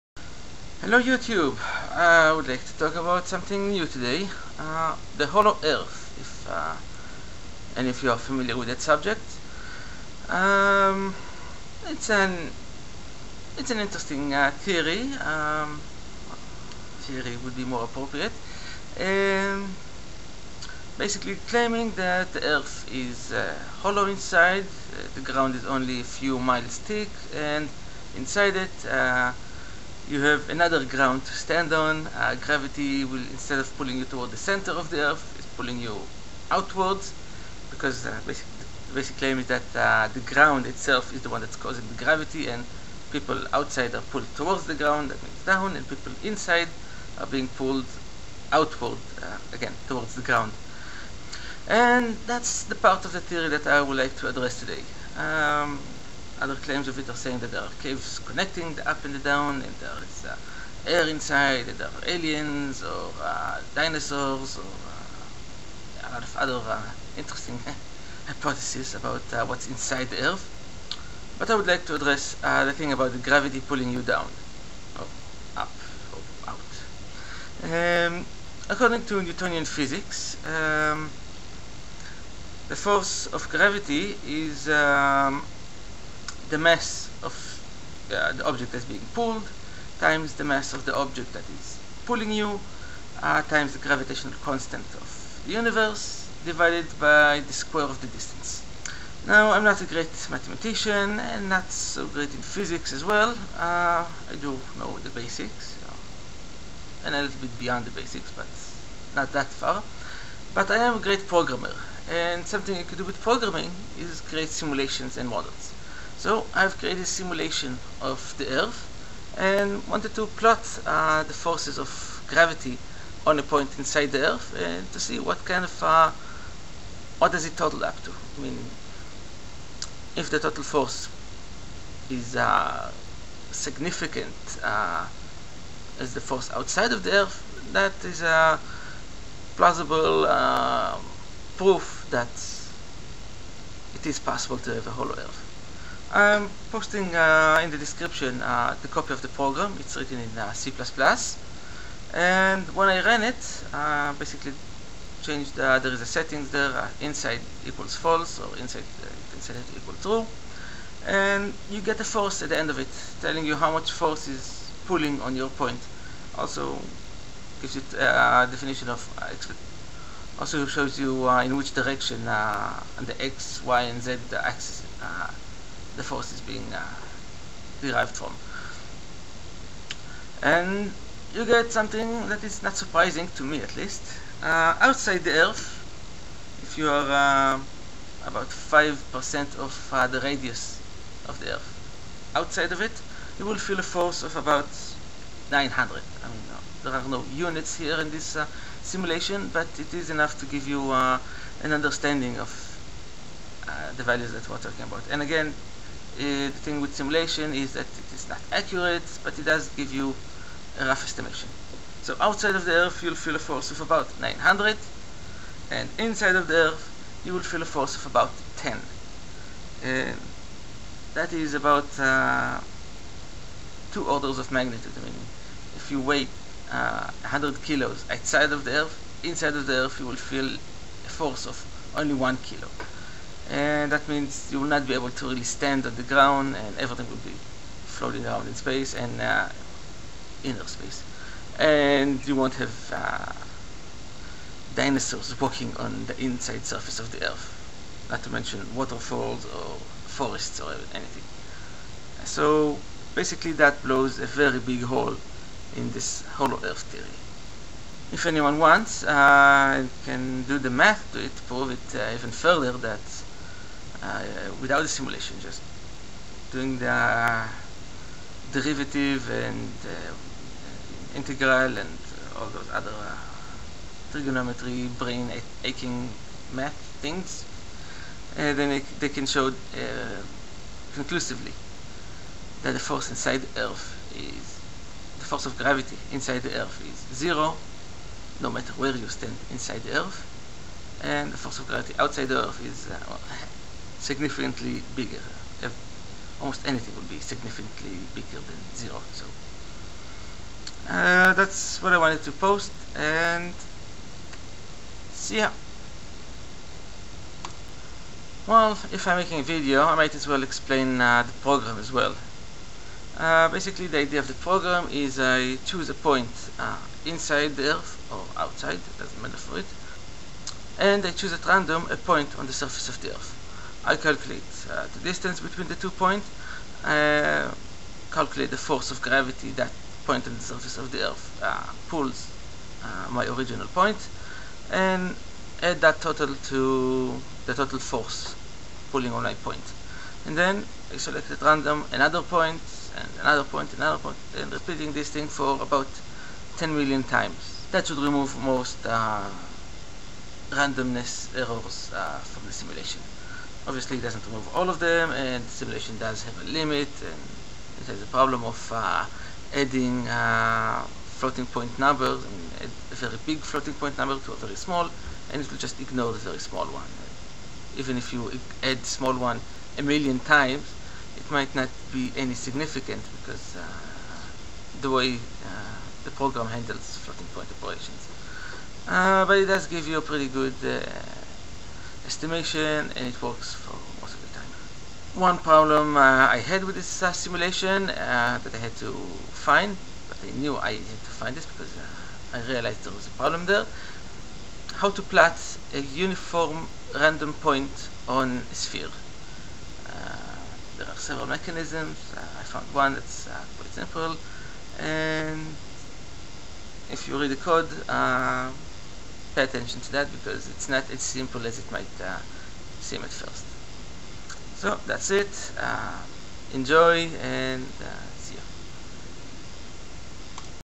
drilling holes in the hollow earth theory
something I've been thinking about for a while (how to prove that if the earth was hollow, you'd have no force of gravity inside it).  Eventually I figured I can use my programming skills to make a simulation.

Now that I have proven it for my self, I decided I should share this with the world.

here's the cpp program I used (if you have questions about this program, or how to go about an proving this with math, leave a comment, or send me a msg):  please note, that you-tube had some problems letting me upload this source code, so I had to replace:
the less than sign with __LT__
the grater than sign with __GT__

so make the "counter replace" inorder to have this program work for you.

include __LT__stdlib.h__GT__
include __LT__stdio.h__GT__
include __LT__time.h__GT__
include __LT__math.h__GT__

bool inside=false;
double x=  0.0;
double y=  0.0;
double z=  0.0;
double r=  10.;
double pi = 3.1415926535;

void main()
    if (inside)
        z=r*0.95;
    else
        z=r*1.05;

    srand((unsigned)time( NULL ));
    double fZ=0.;
    double fY=0.;
    double fX=0.;
    int T=10000000;                            // 10M runs, enought to get ride of most errors
    for (int i=0; i__LT__T; i++)
        double a=((rand()*pi*2.)/RAND_MAX);    // find a random point on the sphere
        double u=((rand()*2.)/RAND_MAX) - 1.;  //
        double tz=r*u;                         // google: random point on a sphere
        double ty=r*sin(a)*sqrt(1-pow(u,2));   // to understand the logic behind
        double tx=r*cos(a)*sqrt(1-pow(u,2));   // this math
        tz=tz-z;
        ty=ty-y;
        tx=tx-x;
        double d=sqrt(pow(tz, 2) + pow(ty, 2) + pow(tx, 2));   // finding the distance between "us" and the point
        double f=(100000./(pow(d, 2)*T));                      // the force we "feel" from this point
        double c=f/d;                                          // ratio between the two "triangles"
        fZ+=tz*c;                                              // add the force parts to total force parts
        fY+=ty*c;
        fX+=tx*c;
    printf("forces:\nX:%3.5f\nY:%3.5f\nZ:%3.5f\nF=%3.5f\n",fX, fY, fZ, sqrt(pow(fX, 2) + pow(fY, 2) + pow(fZ, 2)));  // print result